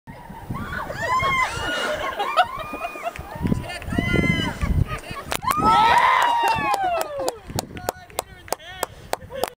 Michelle's Bridge Jump
Michelle jumping from a bridge over the Salmon river.